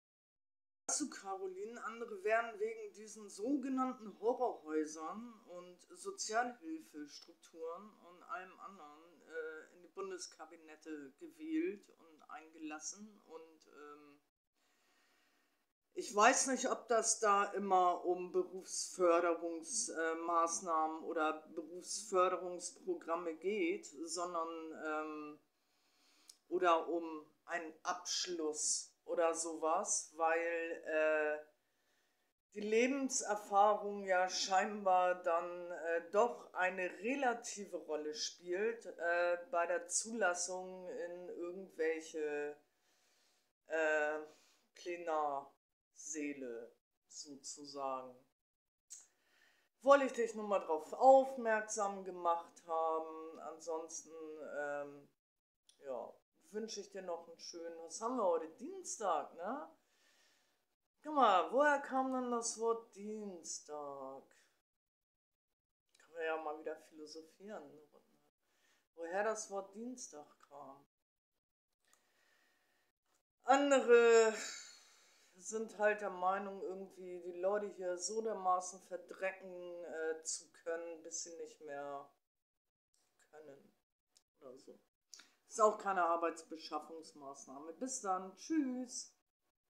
Videoantwort Carolin Matthie.
Auch Nebenberufliche, Rentner und Andere ohne Abschluss können sich in Lokalpolitik einschreiben oder in die Bundestage gewählt werden.

Nur mal so zur Information.